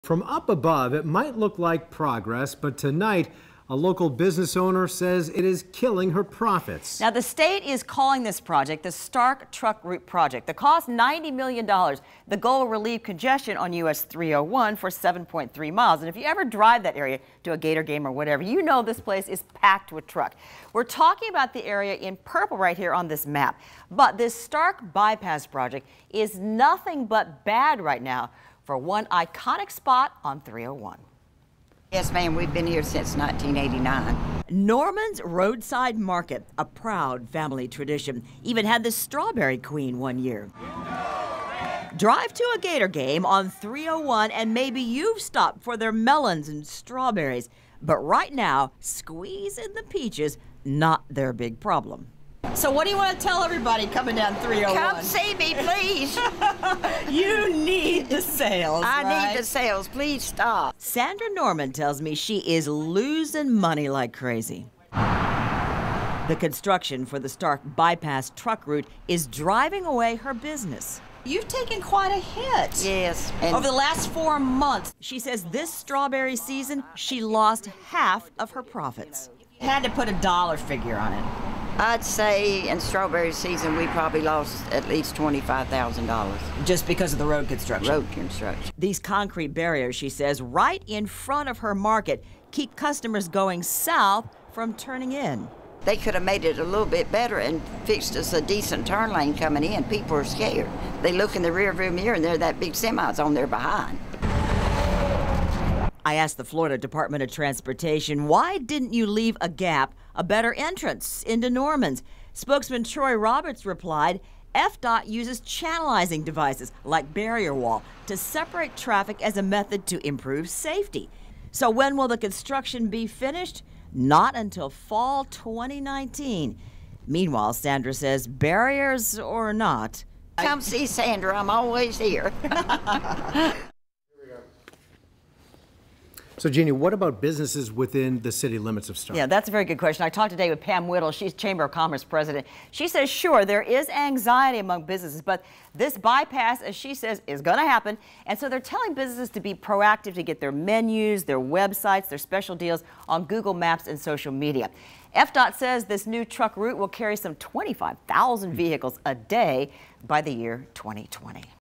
Business owner says Starke Bypass project cost her $25,000
Norman's Produce Market on U.S. 301 has been selling strawberries, local honey, and fresh veggies since 1989. Owner Sandra Norman says this strawberry season was awful.